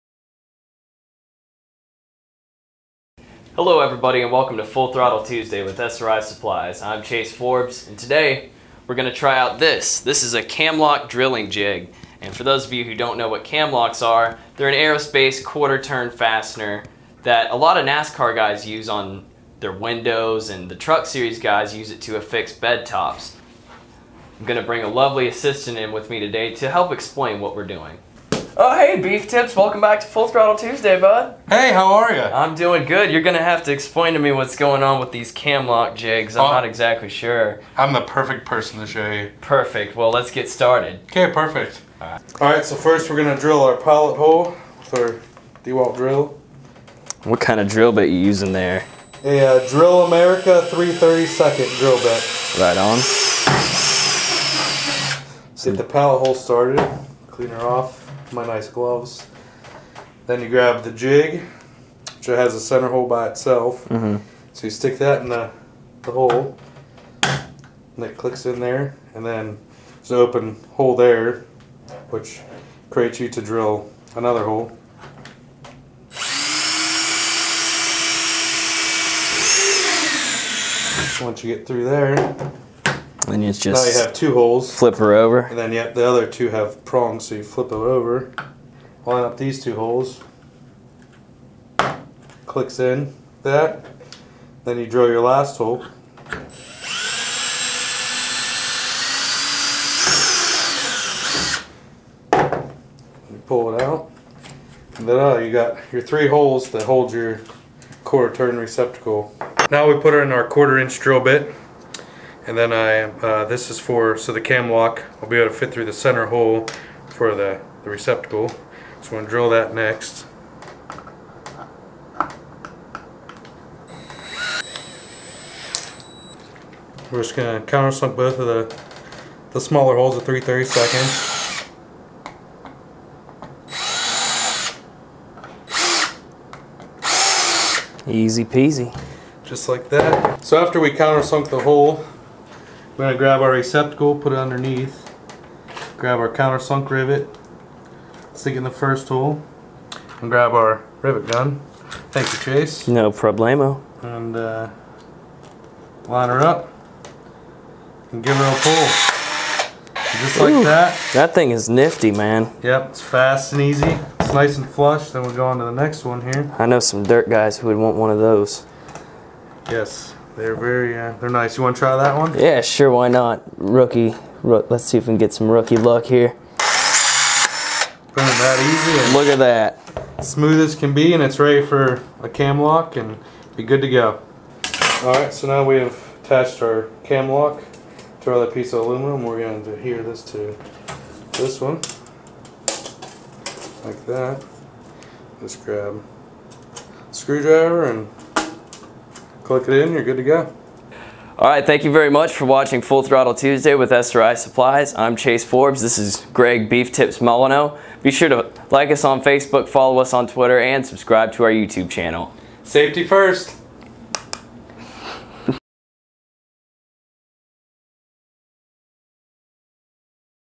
SRI Supplies Presents: How to Install Camlocs
This week, we use a specialty tool designed specifically for drilling to install camlocs, which are quarter-turn aerospace-grade fasteners. We also bust out a cordless battery-powered rivet gun that makes riveting a breeze! Greg "Beeftips" Molyneaux guest stars.